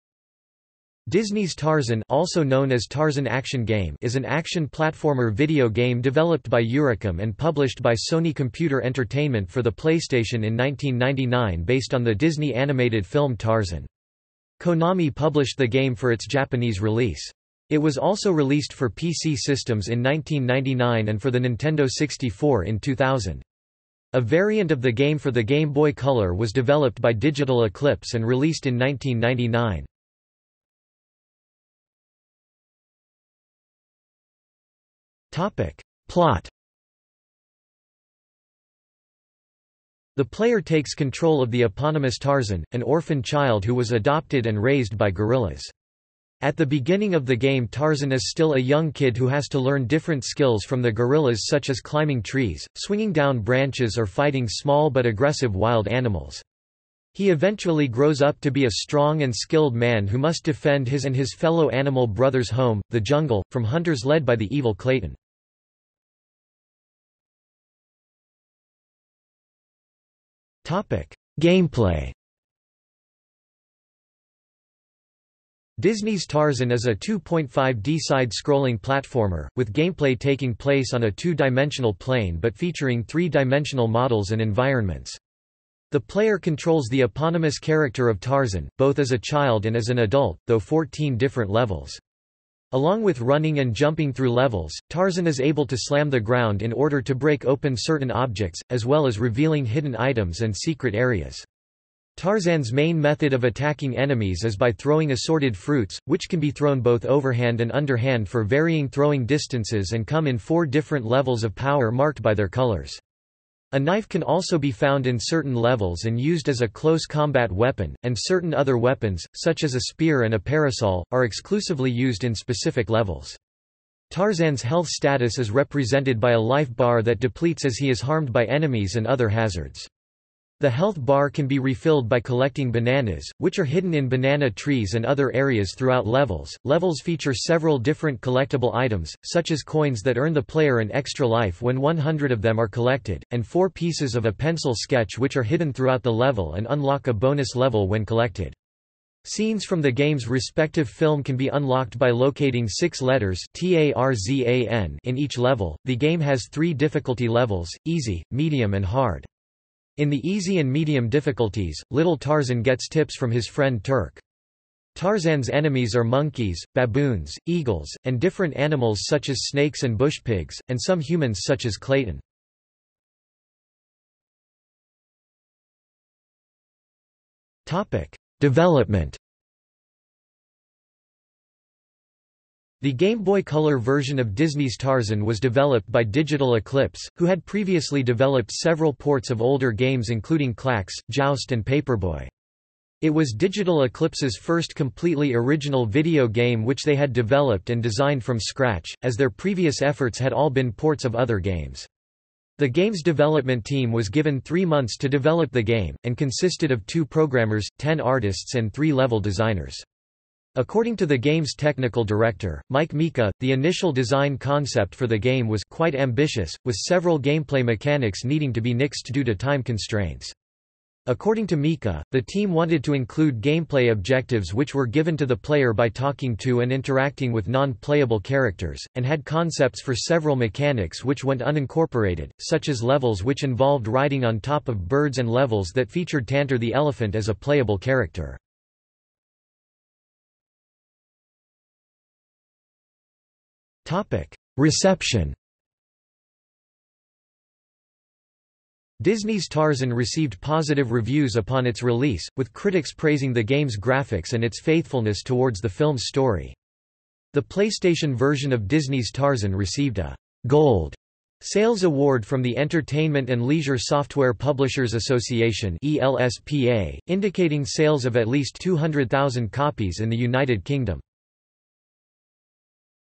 Disney's Tarzan (video game) | Wikipedia audio article
00:00:31 1 Plot
00:01:07 2 Gameplay
00:02:54 3 Development
00:04:00 4 Reception

- Socrates

SUMMARY
Disney's Tarzan (also known as Tarzan Action Game) is an action platformer video game developed by Eurocom and published by Sony Computer Entertainment for the PlayStation in 1999 based on the Disney animated film Tarzan. Konami published the game for its Japanese release. It was also released for PC systems in 1999 and for the Nintendo 64 in 2000. A variant of the game for the Game Boy Color was developed by Digital Eclipse and released in 1999.